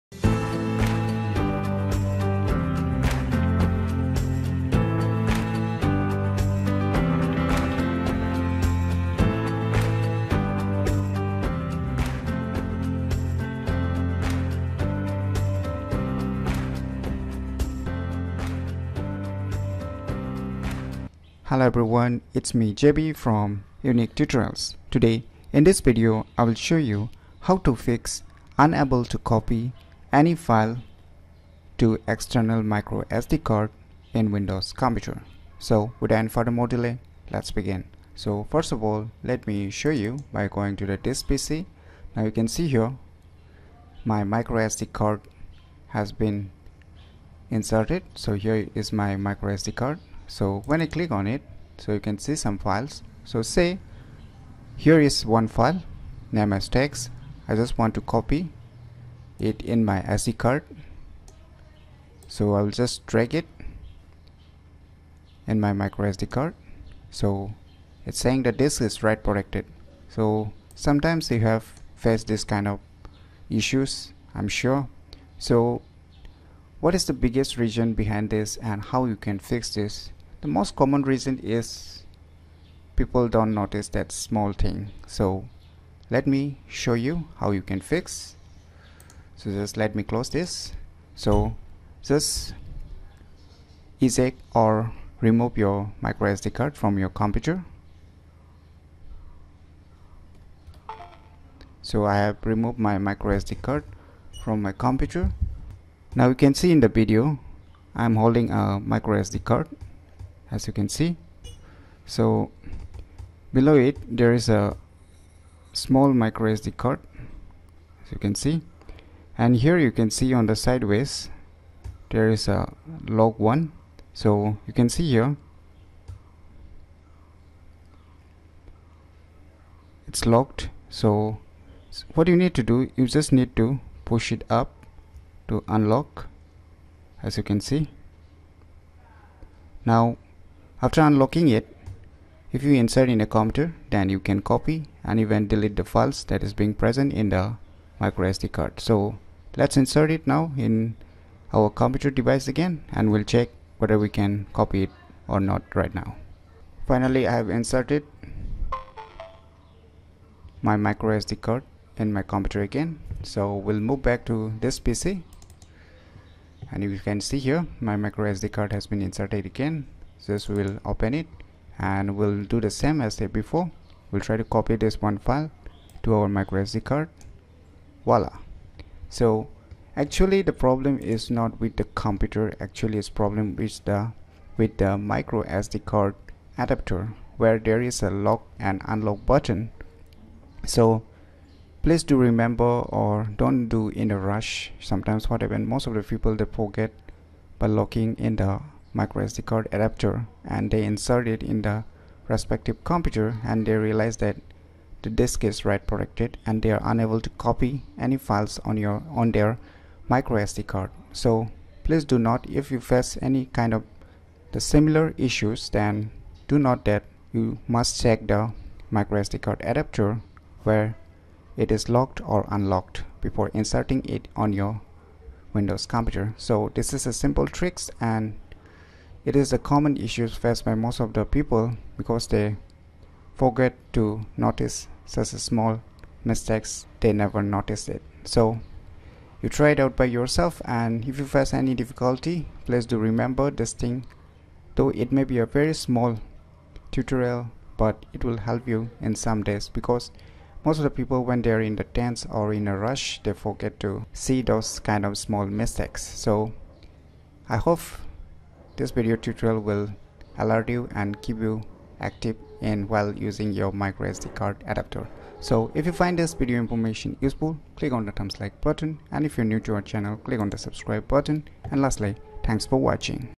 How to Fix unable to copy any files to External Micro SD Card inserted in any Computer ?
This video tutorial is all about How to Fix unable to copy any files to External Micro SD Card inserted in any Computer.